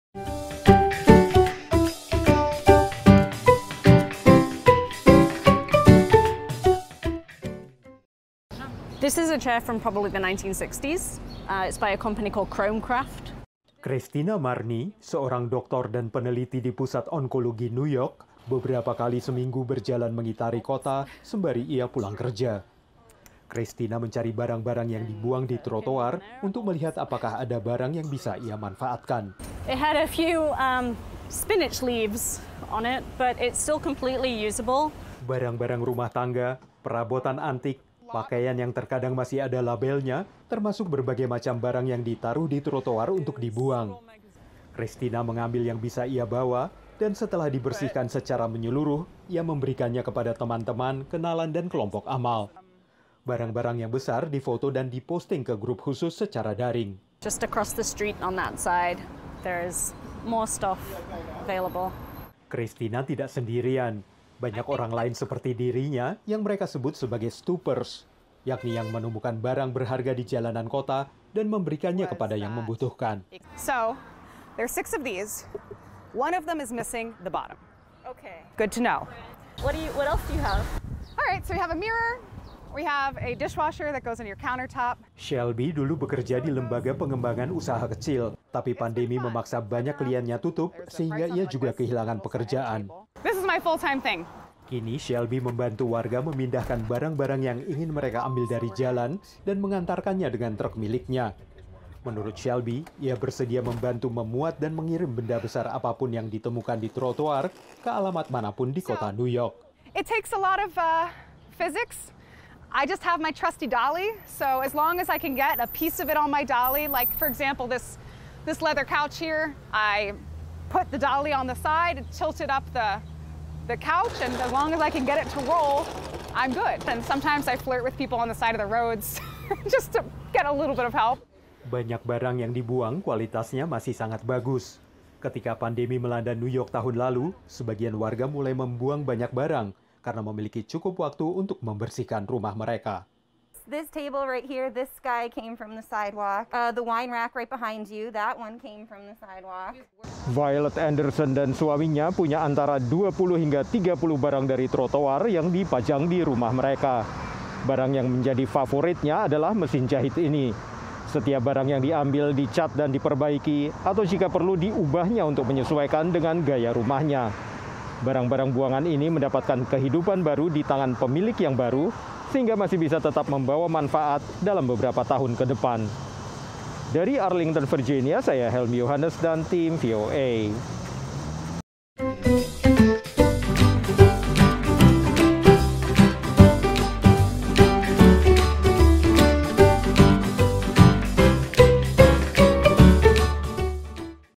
Warga New York Manfaatkan Barang Bekas yang Dibuang di Trotoar
JurnasTV - Sampah seseorang bisa jadi harta bagi orang lain. Itu yang dilakukan sebagian warga kota New York yang mencoba memanfaatkan barang yang dibuang warga lain di trotoar. Barang seperti mebel atau buku dibuang karena tidak dibutuhkan lagi, tapi bagi orang lain masih bermanfaat. Berikut laporan tim VOA.